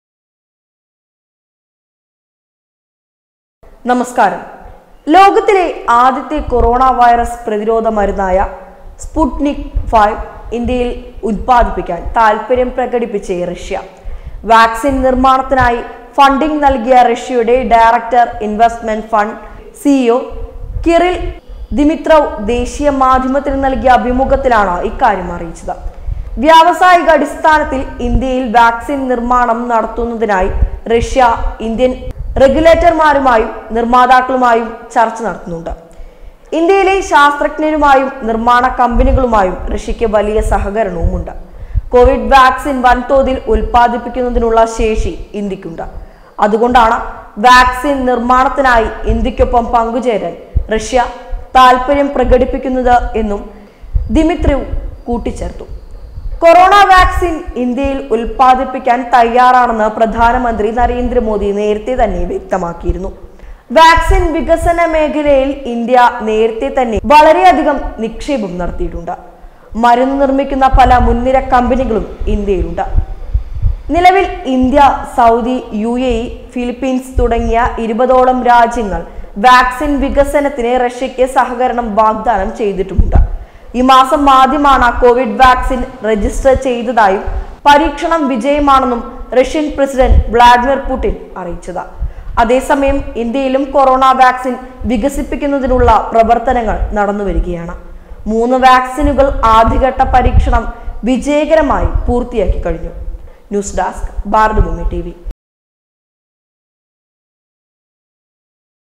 കൊറോണ മരുന്ന് ഇന്ത്യയിൽ നിർമിക്കാൻ റഷ്യ|corona vacssin|india|covidvaccineindia|china
ചൈനയുടെ വൈറസ് യുദ്ധത്തിൽ മറുപണി നൽകാൻ റഷ്യ ഇന്ത്യയിൽ ഇറങ്ങുന്നു